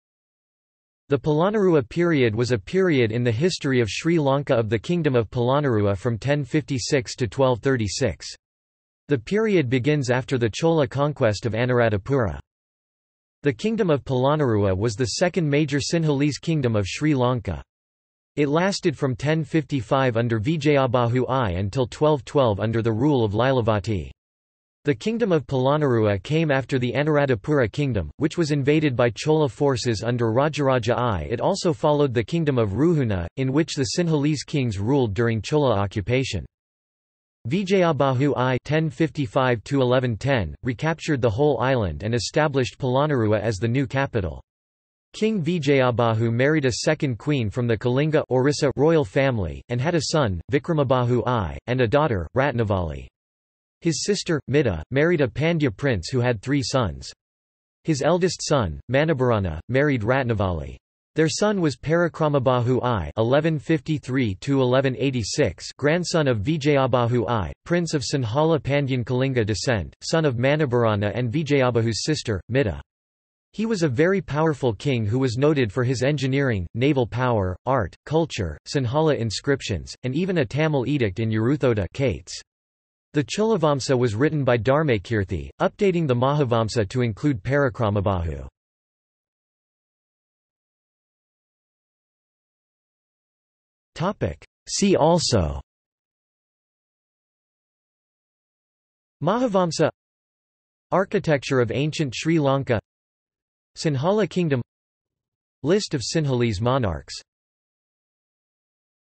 Polonnaruwa period | Wikipedia audio article
This is an audio version of the Wikipedia Article:
Polonnaruwa period

00:01:30 undefined

- Socrates

SUMMARY
The Polonnaruwa period was a period in the history of Sri Lanka of the Kingdom of Polonnaruwa from 1056 to 1236. The period begins after the  Chola conquest of Anuradhapura.
The Kingdom of Polonnaruwa was the second major Sinhalese kingdom of Sri Lanka. It lasted from 1055 under Vijayabahu I until 1212 under the rule of Lilavati. The Kingdom of Polonnaruwa came after the Anuradhapura Kingdom, which was invaded by Chola forces under Rajaraja I. It also followed the Kingdom of Ruhuna, in which the Sinhalese Kings ruled during Chola occupation.
Vijayabahu I (1055–1110), recaptured the whole Island and established Polonnaruwa as the new capital. King Vijayabahu married a second queen from the Kalinga (Orissa) Royal Family, and had a son, Vikramabâhu I, and a daughter, Ratnavali. His sister, Mitta, married a Pandya Prince who had three sons. His eldest son, Manabharana, married Ratnavali. Their son was Parākramabāhu I (1153–1186) Grandson of Vijayabahu I, Prince of Sinhala-Pandyan-Kalinga descent, son of Manabharana and Vijayabahu’s sister, Mitta. He was a very powerful king who was noted for his engineering, naval power, art, culture, Sinhala inscriptions, and even a Tamil edict in Uruthota (Kayts). The Chulavamsa was written by Dharmakirthi, updating the Mahavamsa to include Parakramabahu.